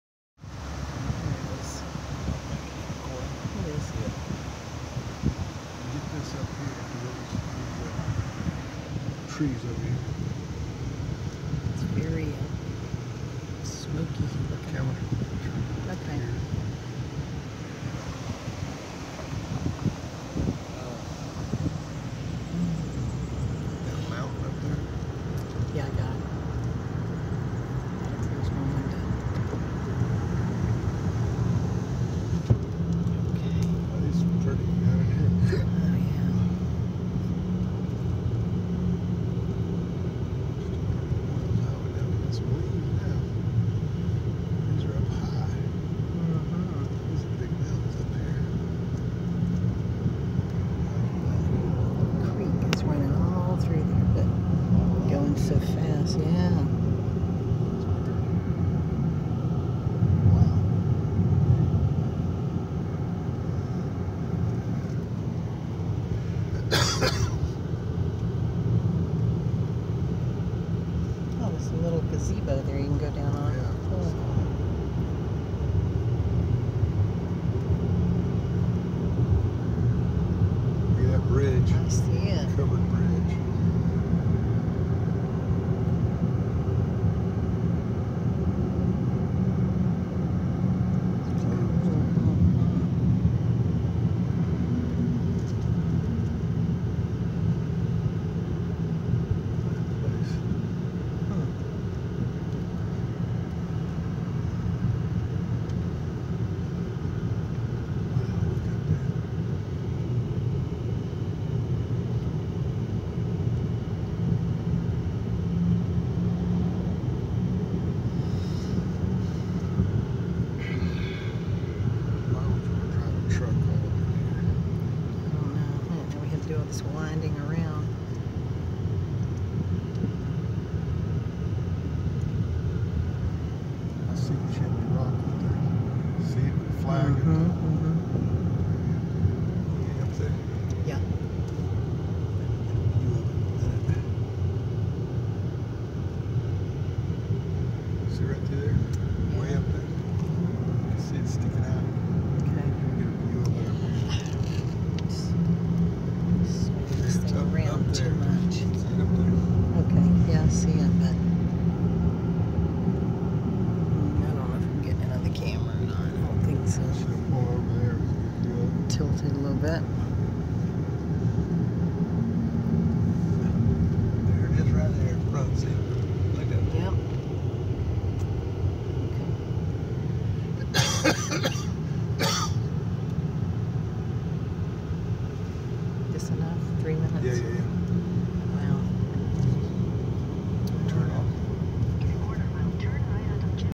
North Carolina mountains
Recent trip to see the changing leaves...flew into Ashville for a three day getaway.